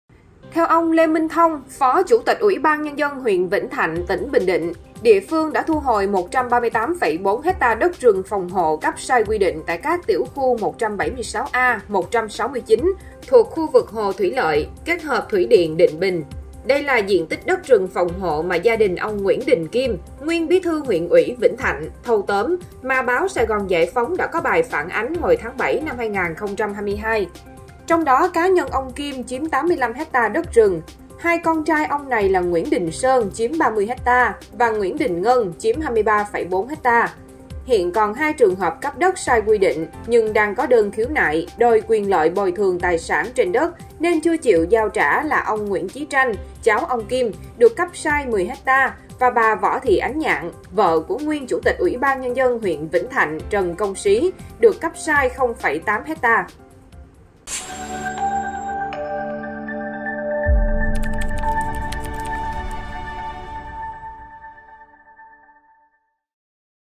Đã thu hồi hơn 138ha đất rừng phòng hộ bị nguyên bí thư huyện “thâu tóm”
Theo Ông Lê Minh Thông, Phó Chủ tịch UBND huyện Vĩnh Thạnh (tỉnh Bình Định), địa phương đã thu hồi 138,4ha đất rừng phòng hộ cấp sai quy định tại các tiểu khu 176a, 169 thuộc khu vực hồ thủy lợi kết hợp thủy điện Định Bình.